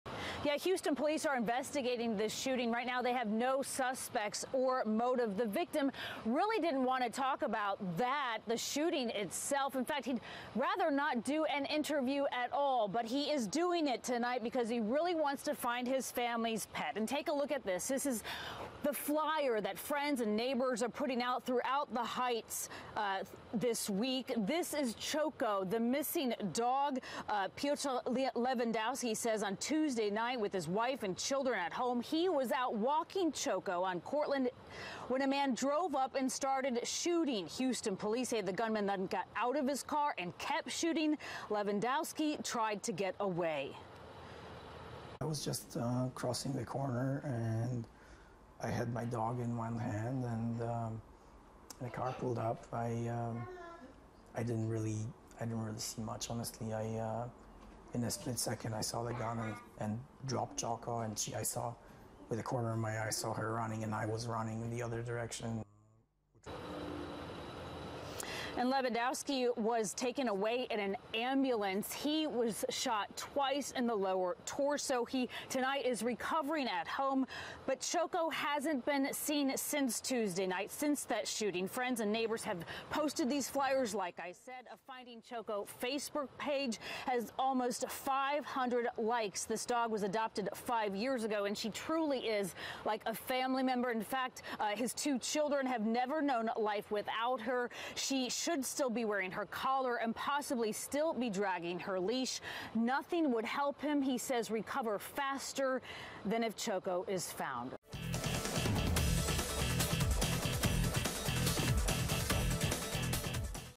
Beloved dog runs away after owner shot in The Heights
WHERE IS CHOCO? Piotr Lewandowski's beloved dog went missing after someone opened fire on them while they were out for a walk in The Heights.